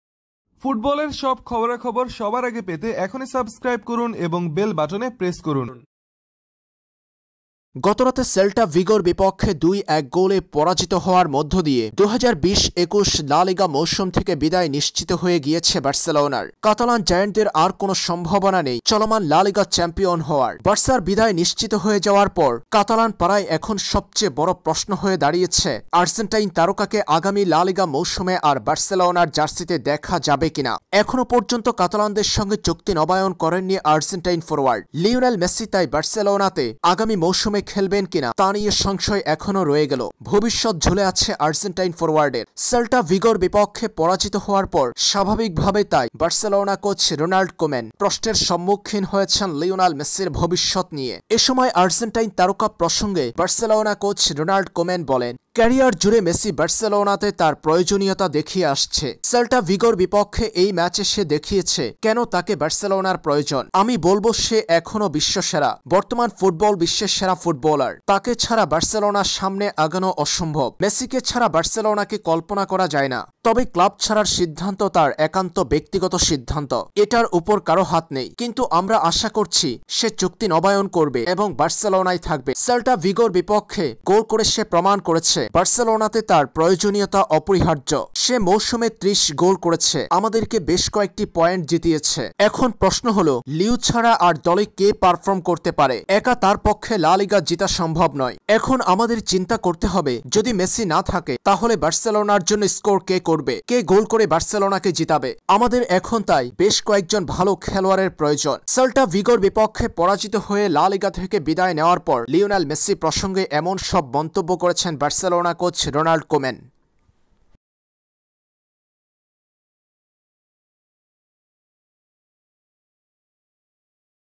মেসি কি বার্সা ছাড়ছেন?? লা লিগা থেকে বার্সার বিদায়ের পর মেসিকে নিয়ে একি বলল বার্সা কোচ | Messi News
Messi transfer news today - fc barcelona news. La Liga 2020/21.
Barcelona vs Celta vigo La liga match 2021.